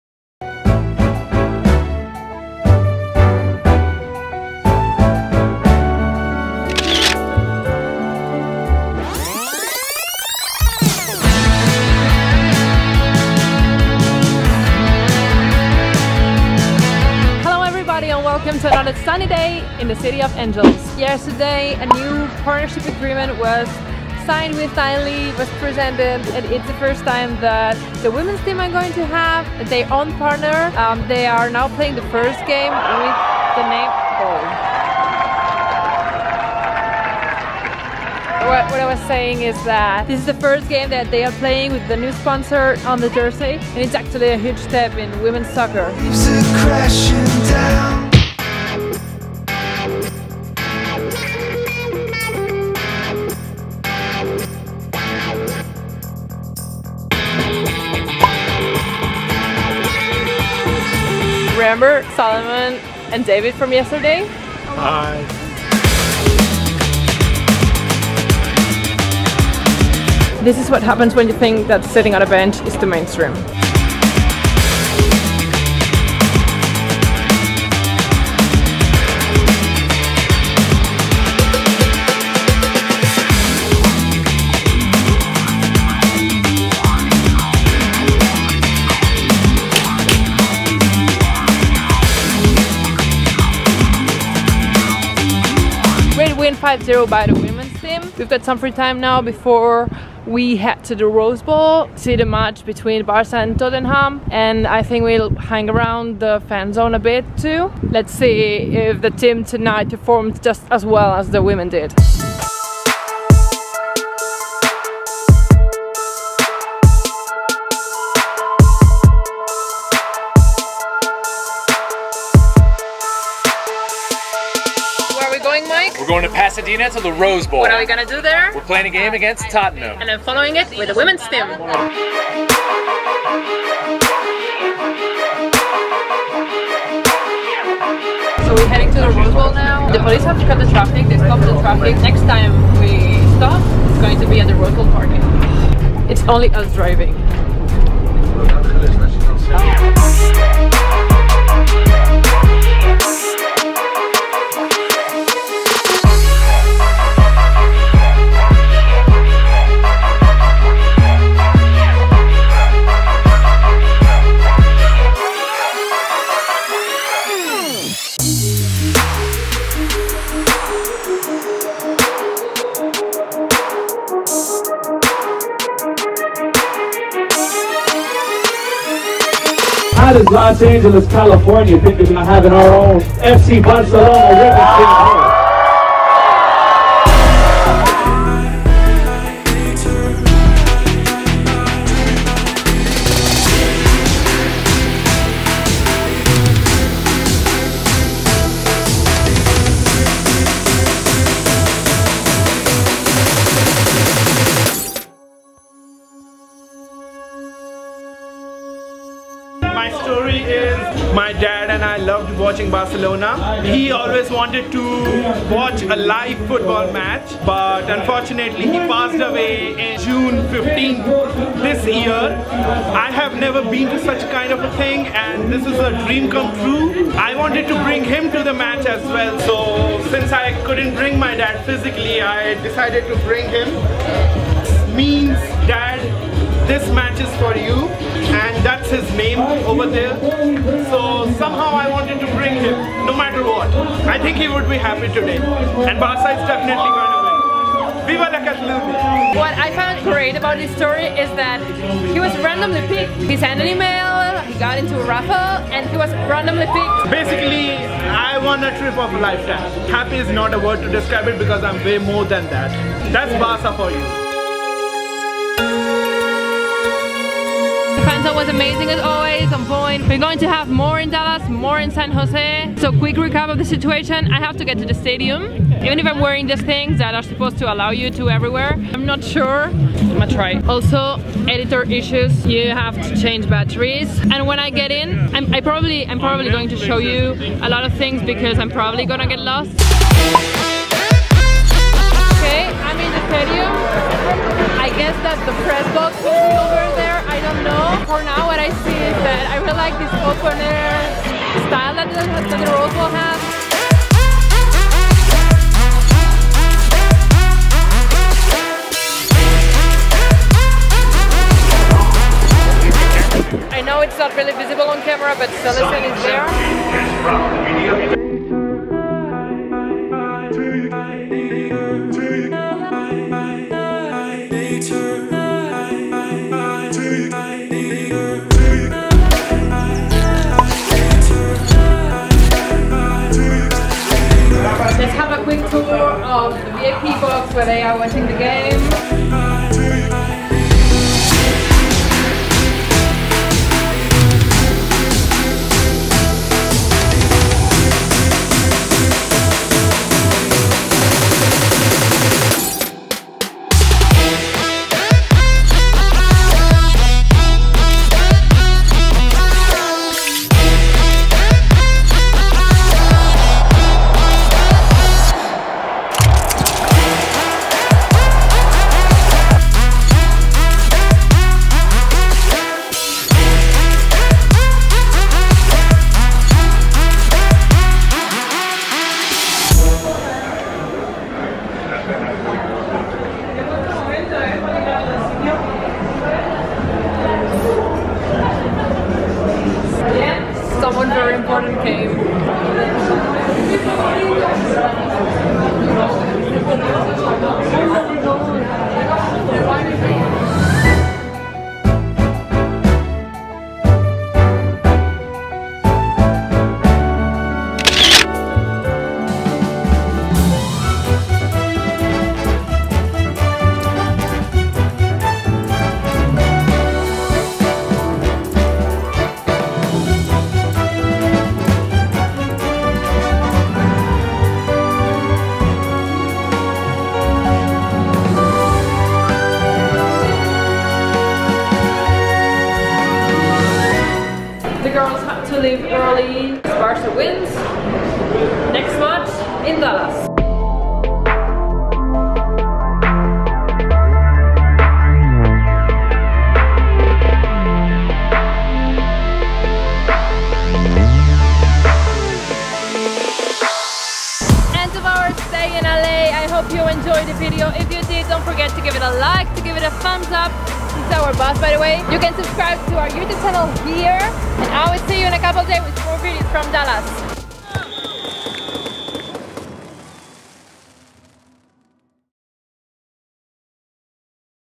Julia Roberts came to our match! VLOG | Vloggin' USA
First match (and win!) of the seson at the Rose Bowl (LA) against Tottenham. And a superstar dropped by...